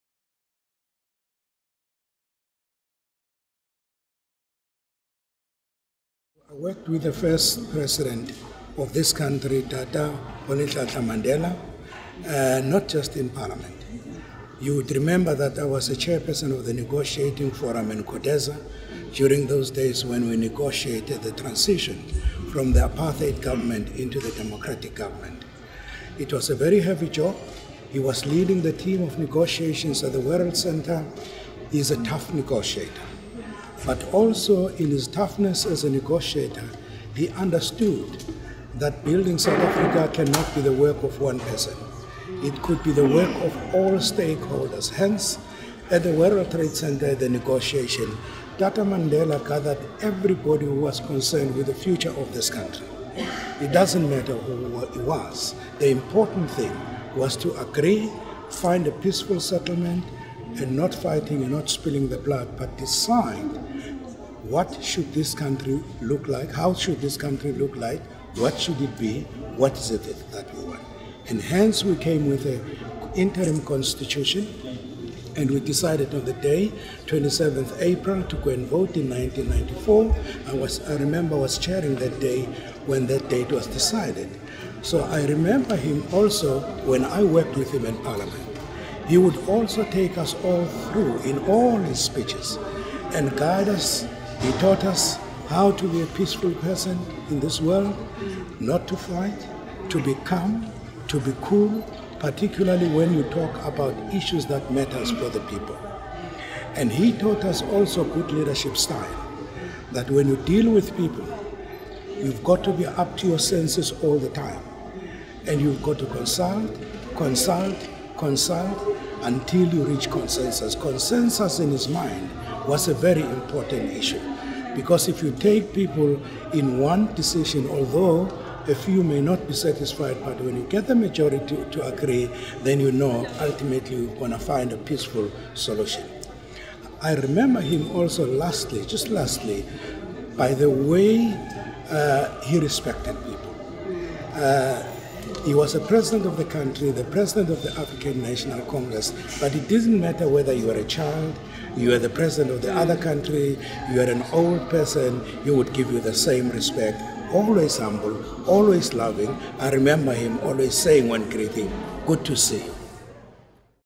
Mr Mninwa Mahlangu pays tribute to Nelson Mandela
9 December 2013
Parliament of the Republic of South Africa